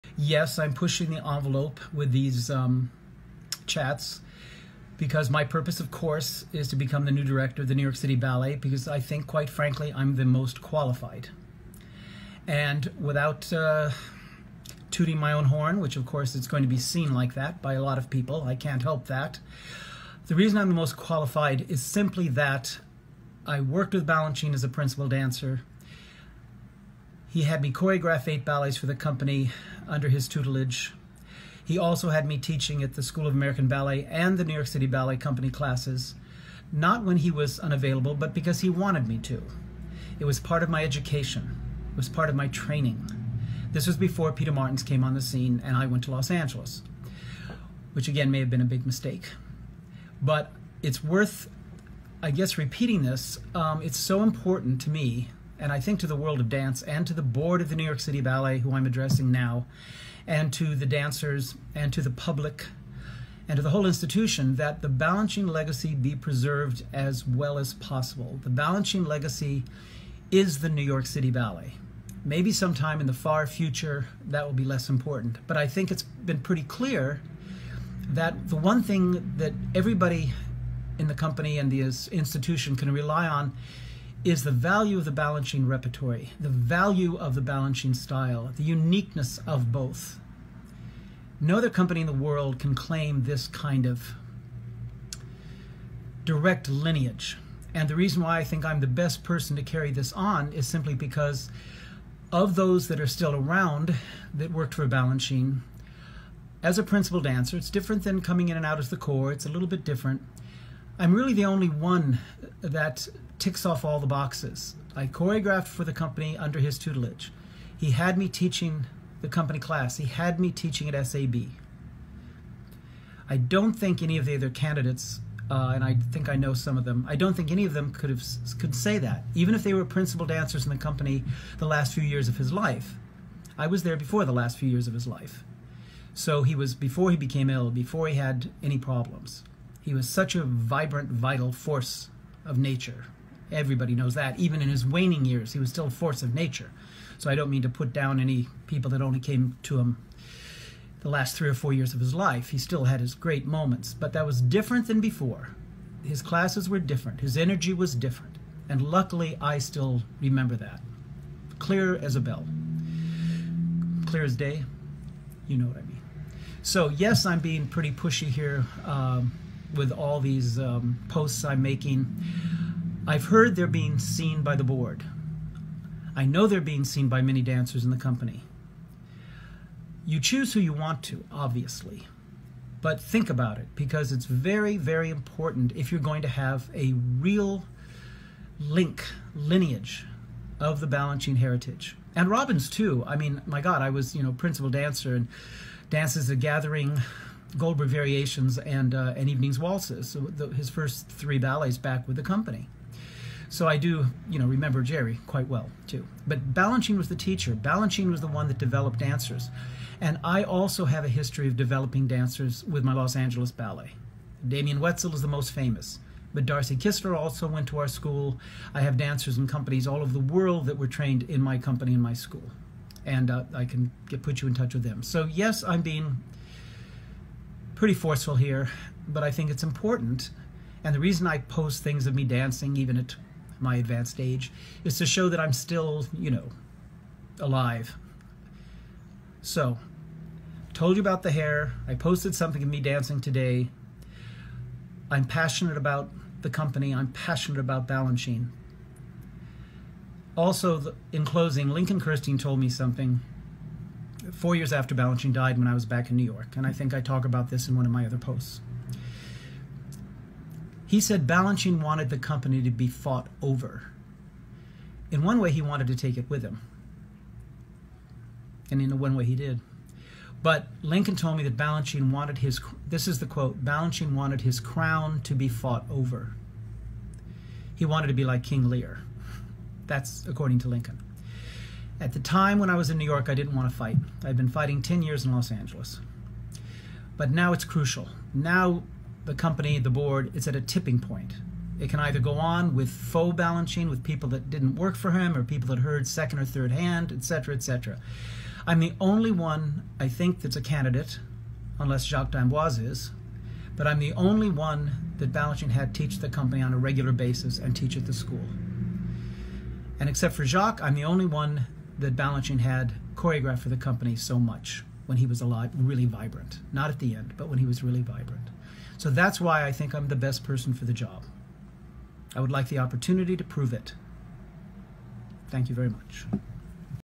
John Clifford on why I believe I'm NYCB's best choice.
Lincoln Kirstein told me “Balanchine wanted his crown to be fought over.” I wasn’t ready when he said this to me 4 years after Mr. B’s death (when he was in a particularly sour mood about Peter Martins) but I wasn’t ready to fight then. Now is a different story.The company (and I believe the Balanchine ballets are the true heart and soul of the company) is way too important. To lose the direct link to Balanchine would be a tragedy. Bringing in McBride, Villella, and others is great, but you need a leader/teacher/pedagog who can truly develop the younger dancers. This person must be there 24/7. That’s the only way it will work.